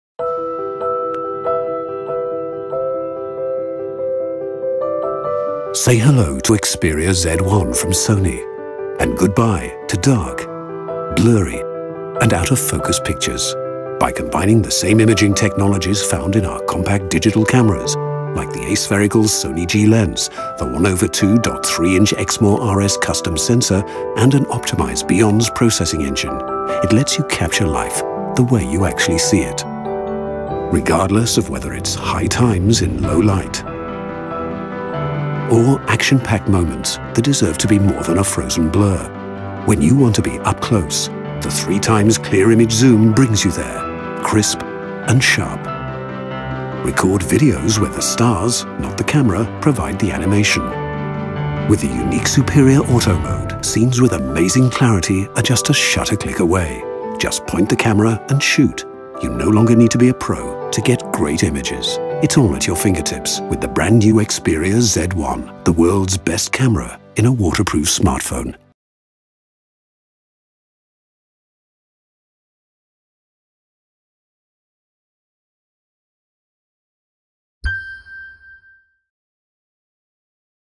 Xperia™ Z1 - The Worlds Best Camera In A Waterproof Smartphone
Say hello to Xperia™ Z1 from Sony, and goodbye to dark, blurry and out of focus pictures. Using the award winning G Lens from Sony, Exmor RS and BIONZ™ processing engine for mobile, the innovative camera captures life the way you actually see it.

Xperia™ Z1 - the best of Sony is now even better.

*The Z1 features a bespoke camera combining the world's largest image sensor (1/2.3 inches) with the highest resolution (20.7MP) in a waterproof smartphone. It also includes the brightest lens (F2.0) available in a slim (8.5mm) waterproof (IP55/IP58) smartphone. Specifications verified by Strategy Analytics' SpecTRAX service as of 5th of August 2013, for more information on Strategy Analytics results go